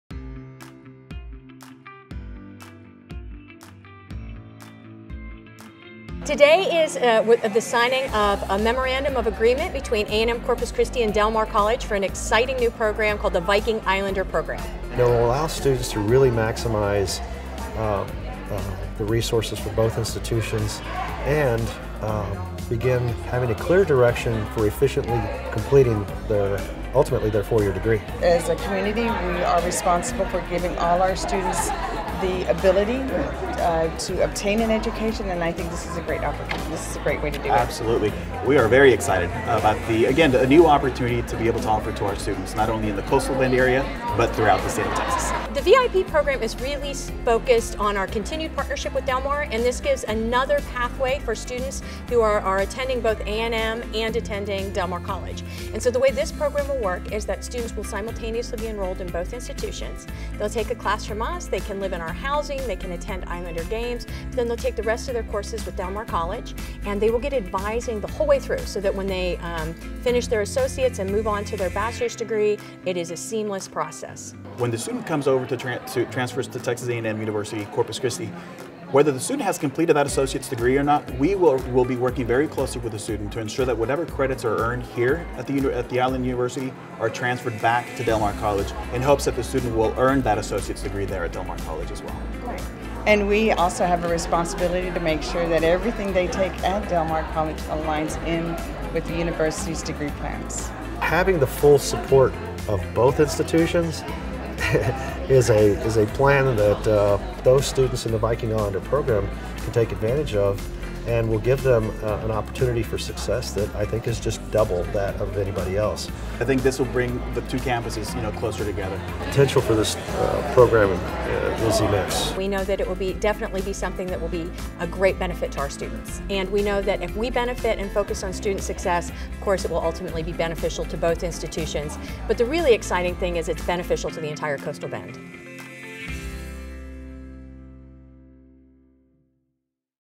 TAMU-CC The new Viking Islander Program VIP
Eligible students can now have all of their “Dreams Delivered” while making an “Islander Impact” thanks to the Viking Islander Program (VIP). The new collaborative co-enrollment partnership between Texas A&M University-Corpus Christi and Del Mar College will provide students with additional tools and support to succeed during their first two semesters of college.